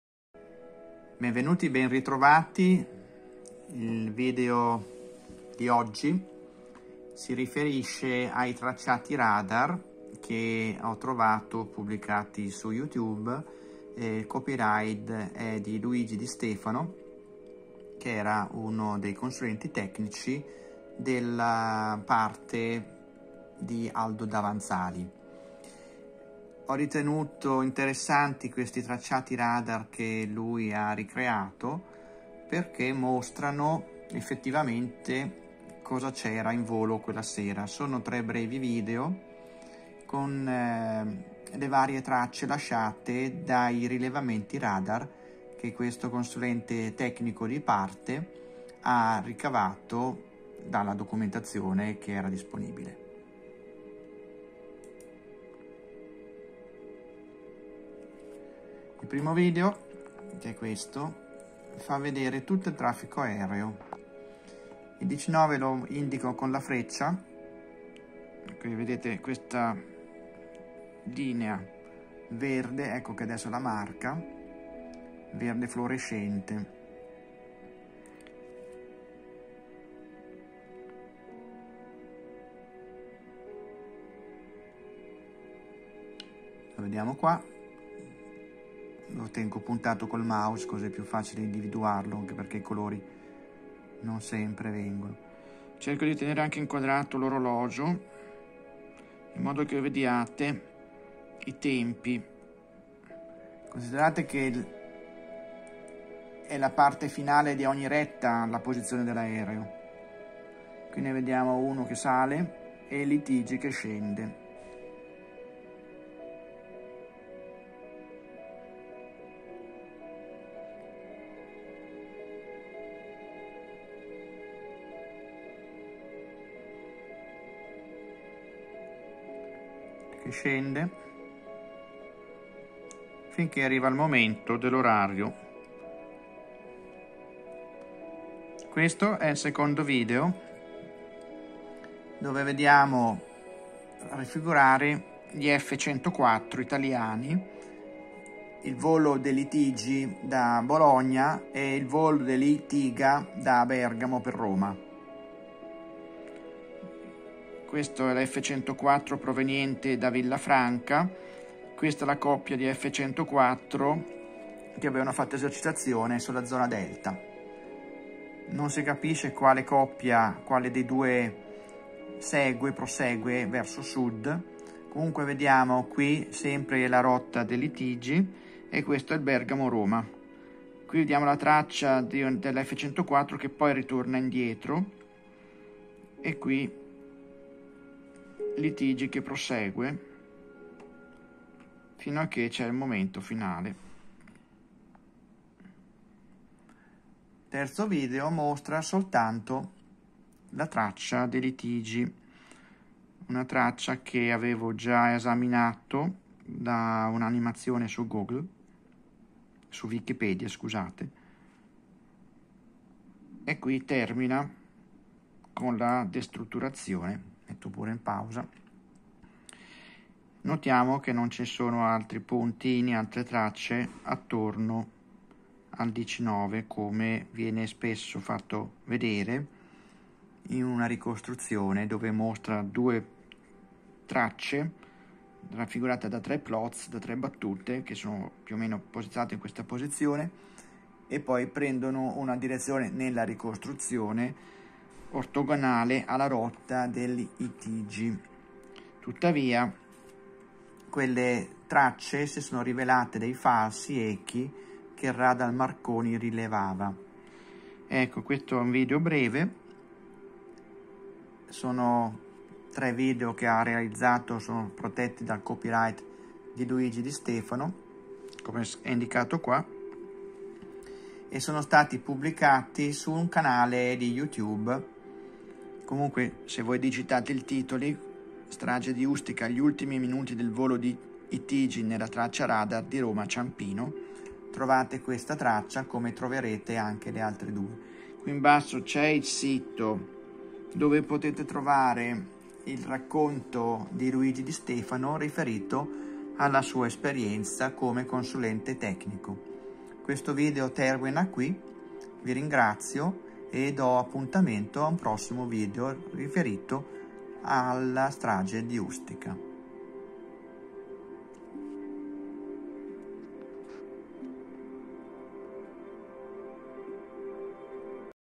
STRAGE DI USTICA RICOSTRUZIONE DEI TRACCIATI RADAR VIDEO 9
Ricostruzione dei tracciati radar effettuata da uno dei periti di parte, Luigi Di Stefano. E' interessante notare come attorno al DC-9 non ci sia nessun altro aereo nei momenti precedenti l'ora dello scoppio.
Alcune rettifiche postume al commento video (ringrazio India Fox Bravo per la segnalazione):
"Al secondo video, la traccia a sinistra NON è quella della coppia di F-104 e l’aereo che si vede sta andando ad atterrare a Pratica di Mare o a Fiumicino. La zona Delta è da tutt’altra parte.
Il 104 di Bergamini che scende da nord si vede che gira per Grosseto molto prima.
Quello che si vede è una ricostruzione dinamica al computer, NON è ciò che gli operatori radar vedevano, ed è riferita a un solo radar civile."
Occorre anche precisare che il perito di Itavia  non era un  esperto né di radar, né di operazioni aeree, né di aeronautica e risulta che millantasse una laurea mai conseguita.